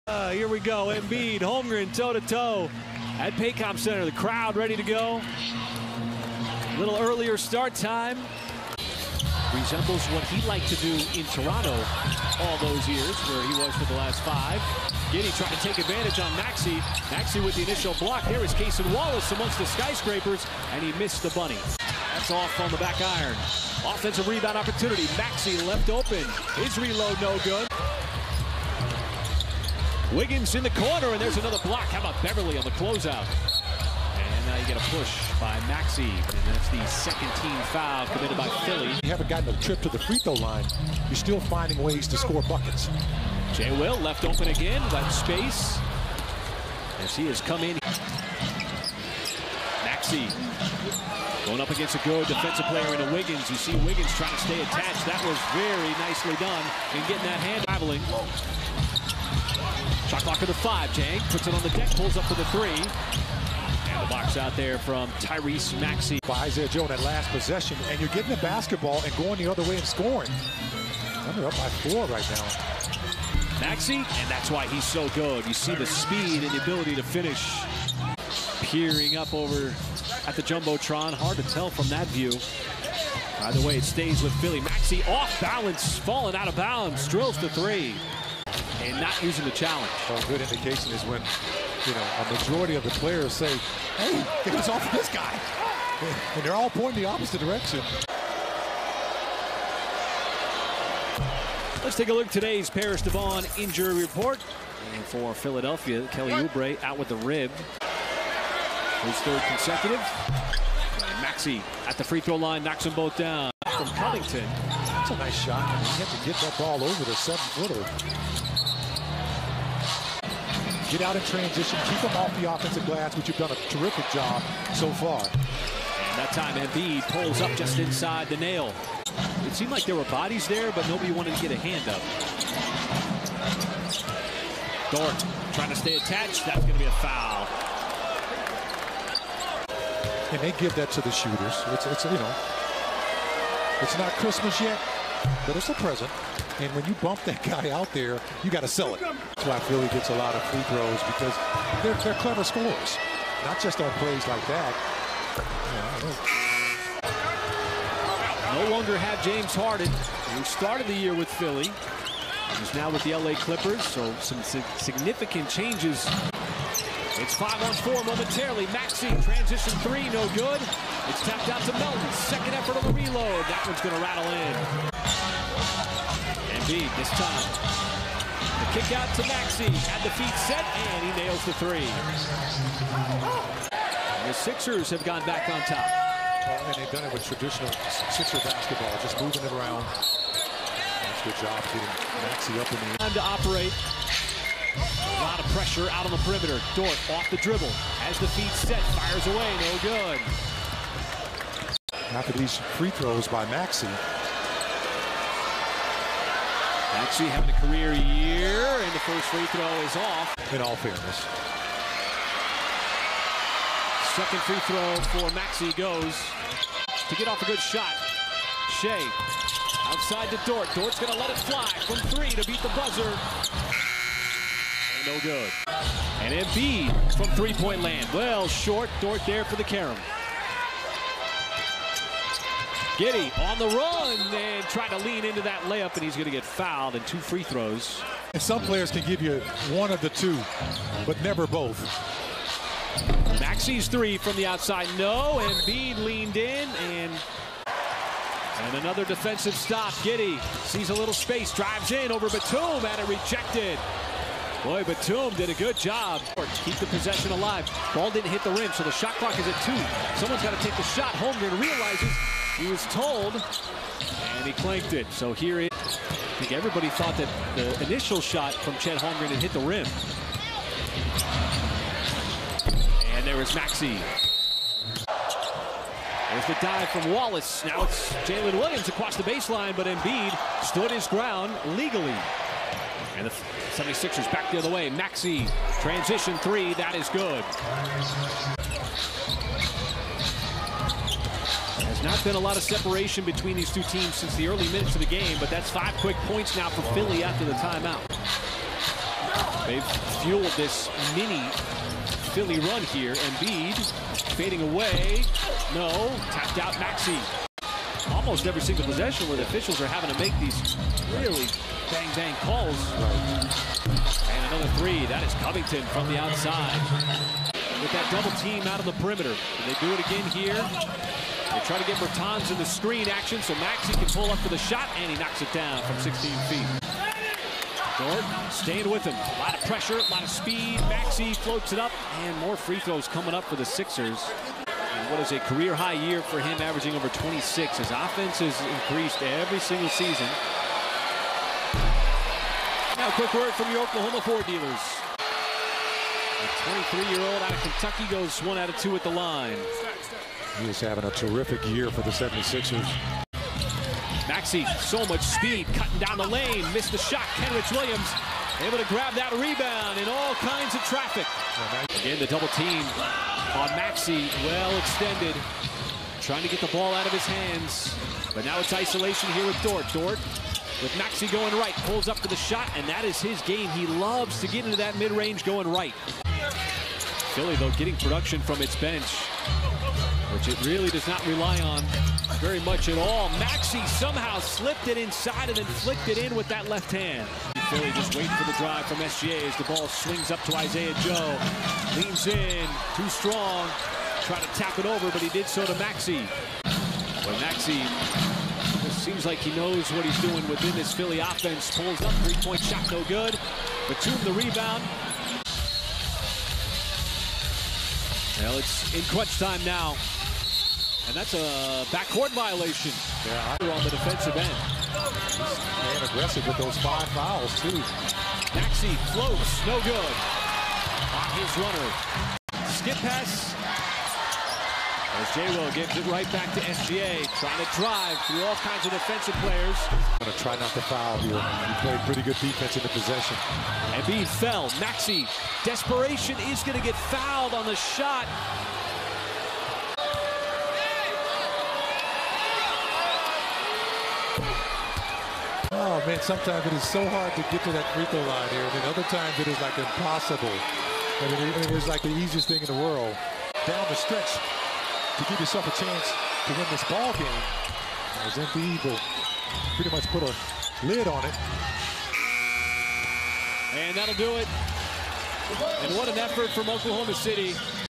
Tyrese Maxey 28 pts 8 reb 3 ast vs Oklahoma City Thunder | 2023-11-25
Tyrese Maxey Highlights from Philadelphia 76ers vs Oklahoma City Thunder on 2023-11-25.

Player Stats:
40:54 mins
28 Points
8 Rebounds (1 Offensive)
3 Assists
0 Steals
1 Blocks
1 Turnovers
8/17 Field Goal
9/11 Free Throw